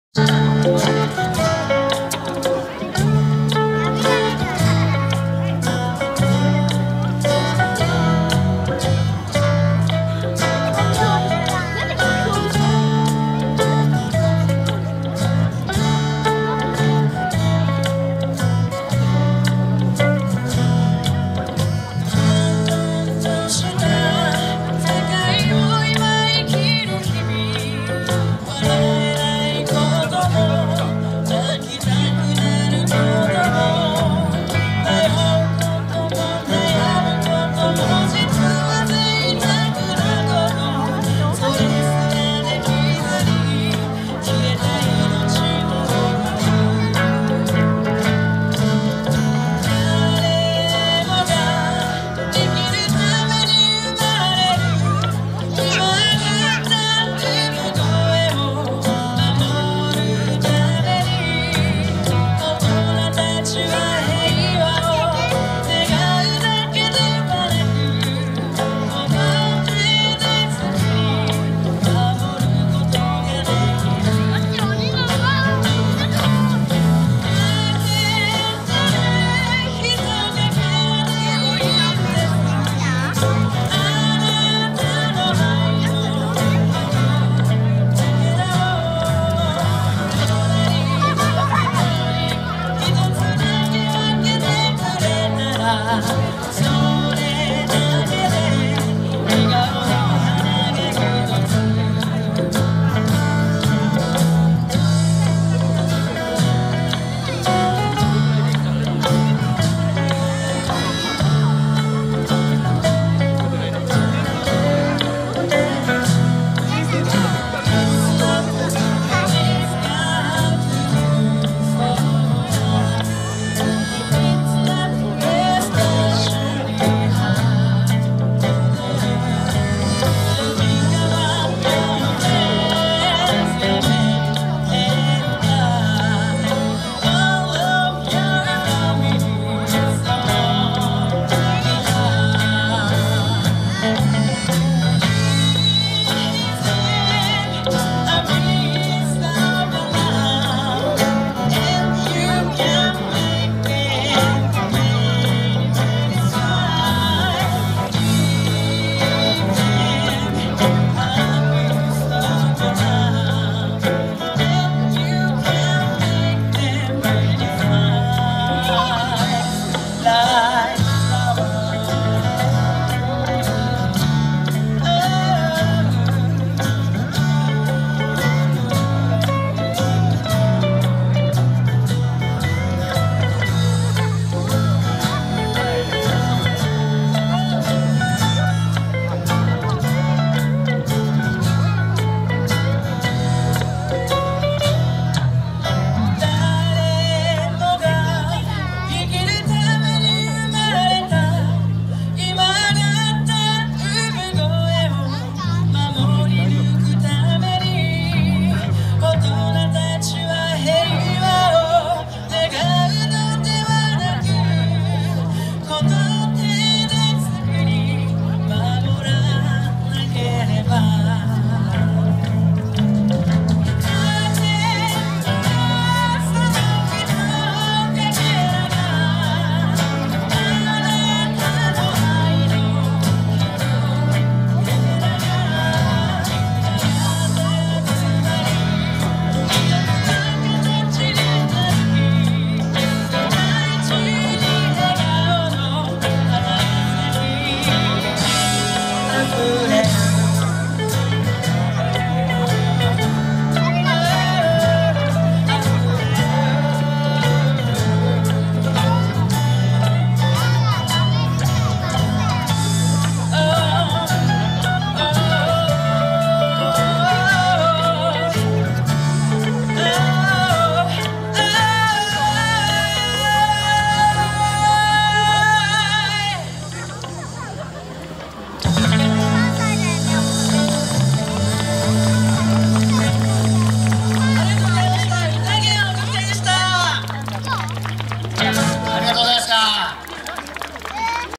2018 亀有ウインターフェスティバル５ バンド演奏５ 宴YA音楽隊５
常磐線亀有駅前亀有リリオパーク会場の、「2018 ウインターフェスティバル」から。